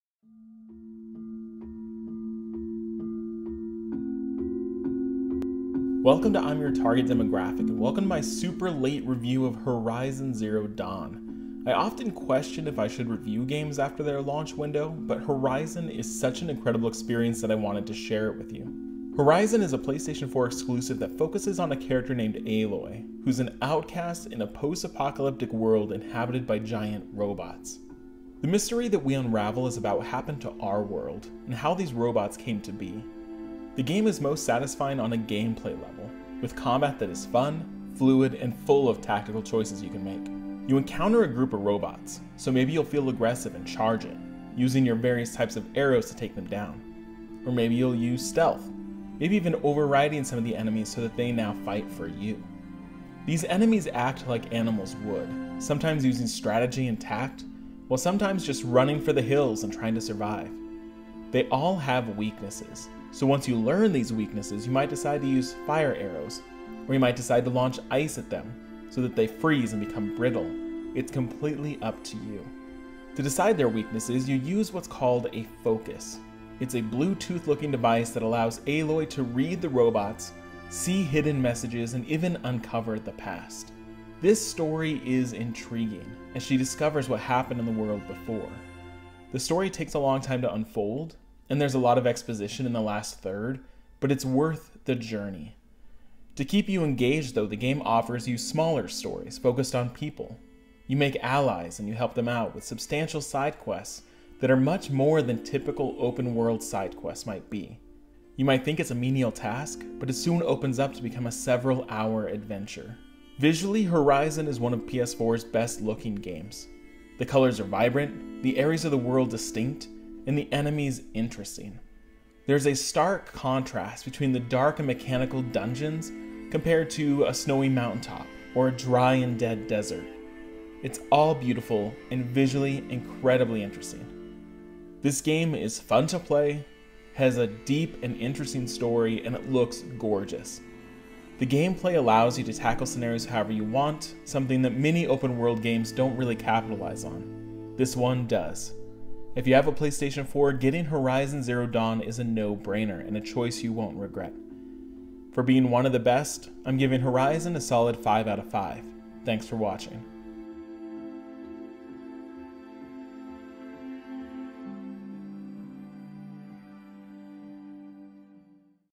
PS4 Review: Horizon Zero Dawn
Here is my belated review for Horizon Zero Dawn, one of PS4's best exclusives!